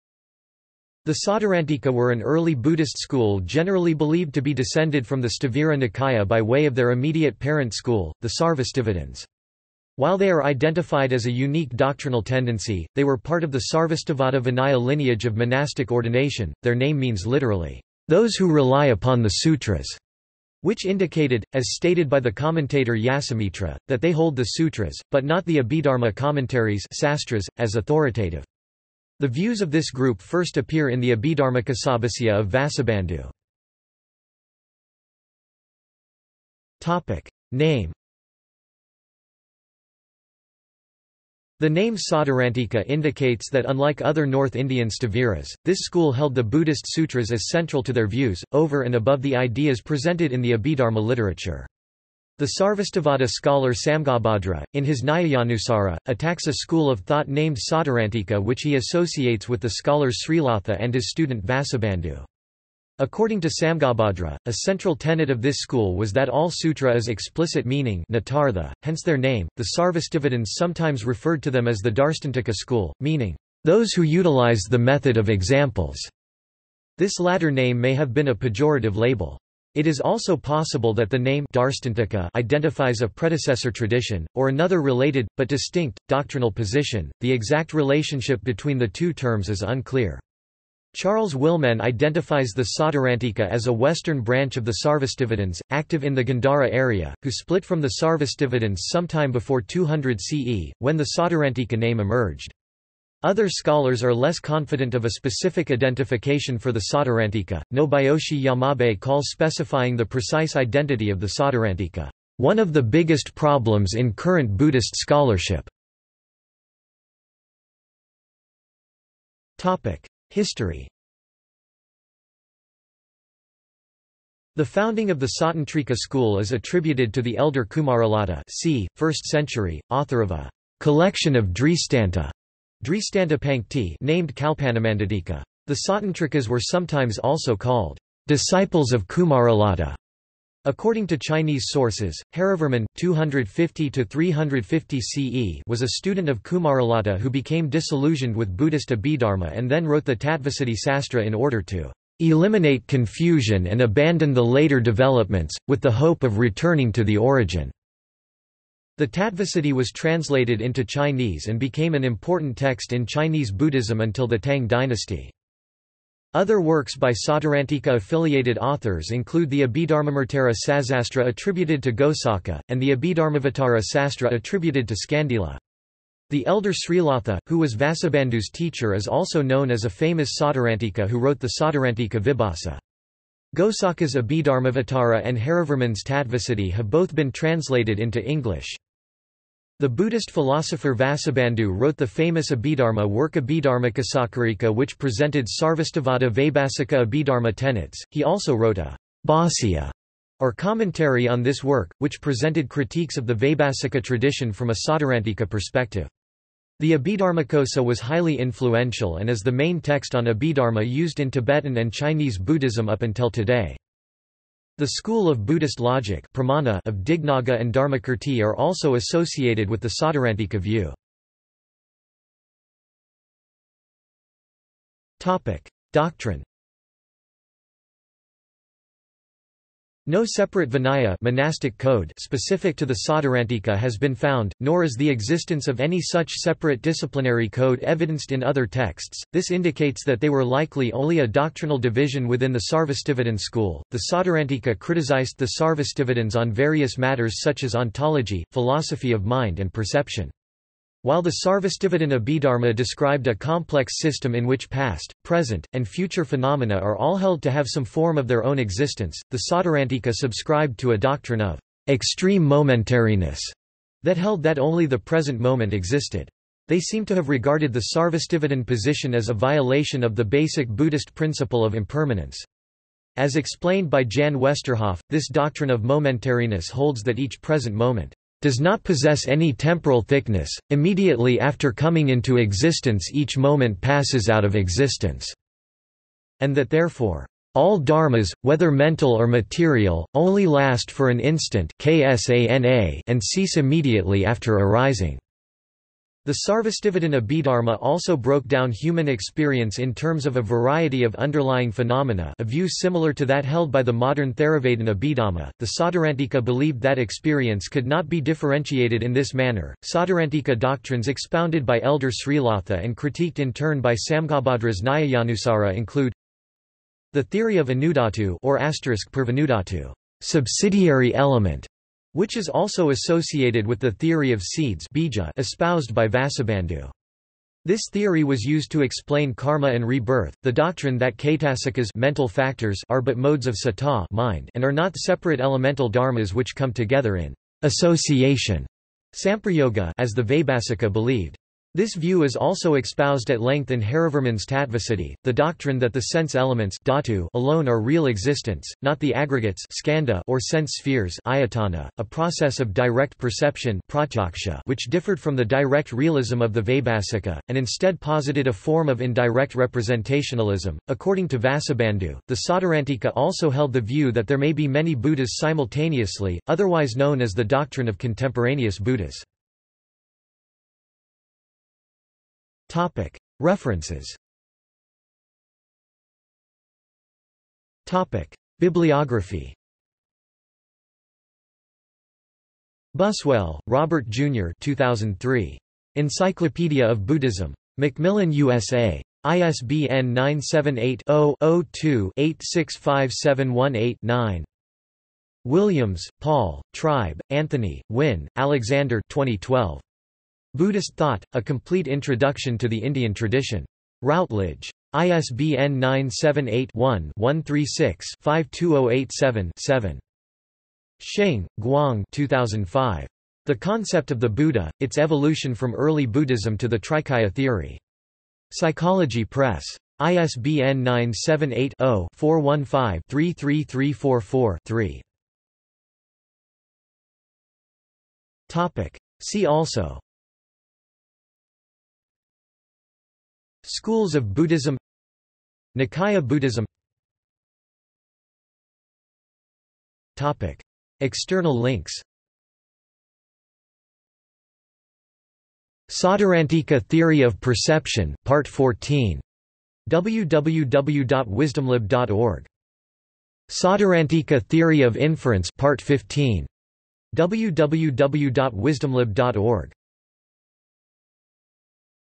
Sautrāntika | Wikipedia audio article
- Socrates

SUMMARY
The Sautrāntika were an early Buddhist school generally believed to be descended from the Sthavira nikāya by way of their immediate parent school, the Sarvāstivādins. While they are identified as a unique doctrinal tendency, they were part of the Sarvāstivāda Vinaya lineage of monastic ordination.Their name means literally "those who rely upon the sutras", which indicated, as stated by the commentator Yasomitra, that they hold the sutras, but not the Abhidharma commentaries (sastras), as authoritative. The views of this group first appear in the Abhidharmakośabhāṣya of Vasubandhu.